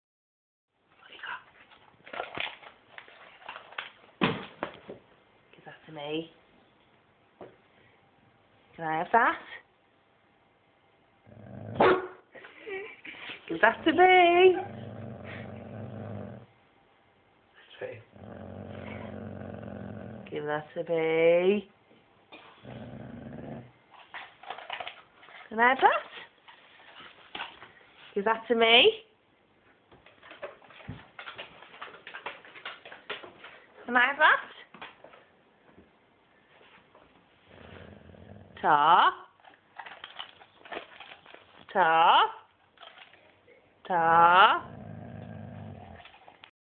Naughty Basset hound pie thief
Coco the Basset hound steals a pie in foil wrapper as nd won't give it up. Takes it to her bed for safe keeping.